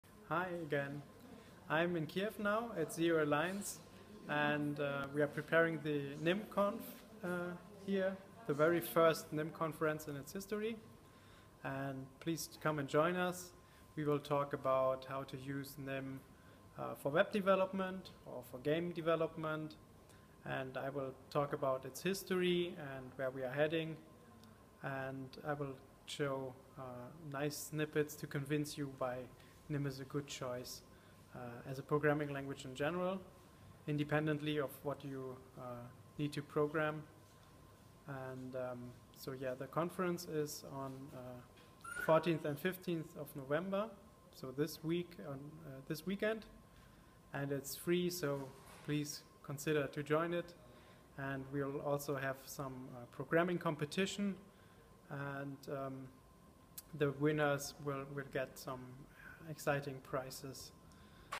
Andreas Rumpf joining the Nim conference
Andreas Rumpf, creator of Nim language, will join the first Nim conference held in Kyiv!